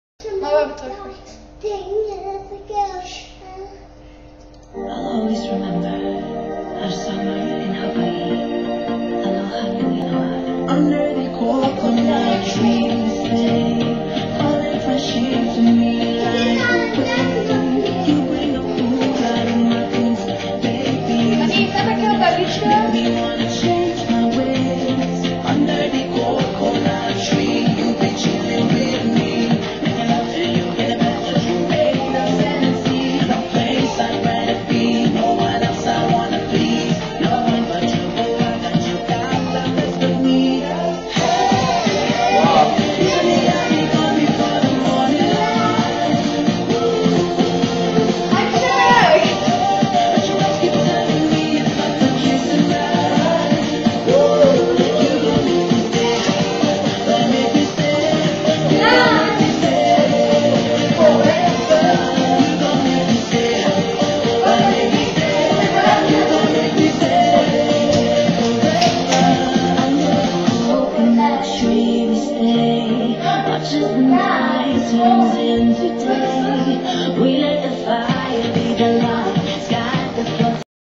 MY CUTE COUSIN DANSING I THINK ITS PRITTY GOOD !
my cousin dancing for fun , i think its pritty good, but shé wanted to be a dancer